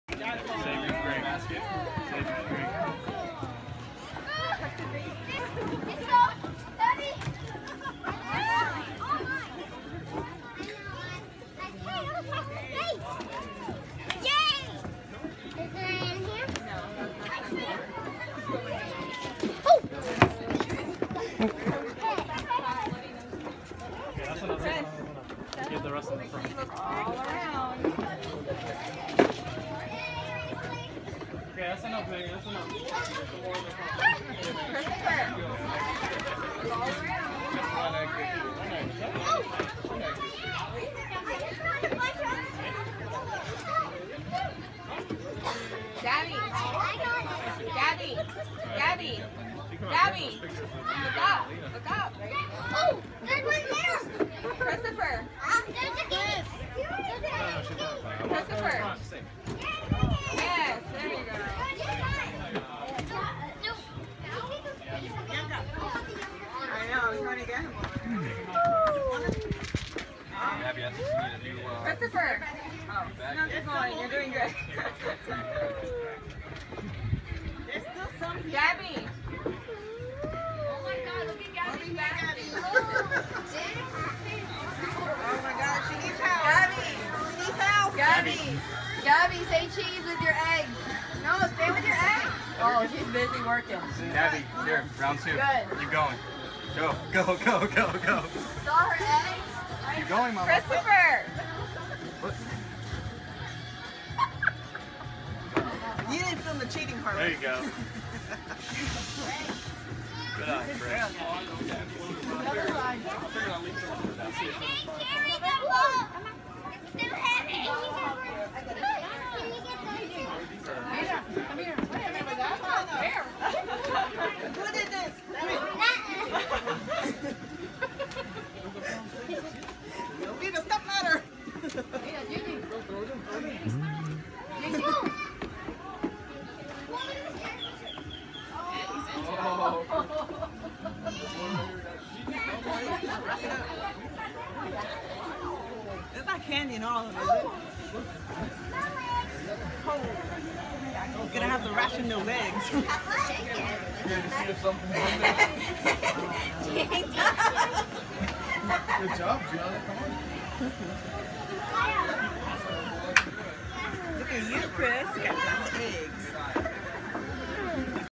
Easter Egg Hunt 2017
Easter Egg Hunt at Gumary and Juan's house. It was a great time.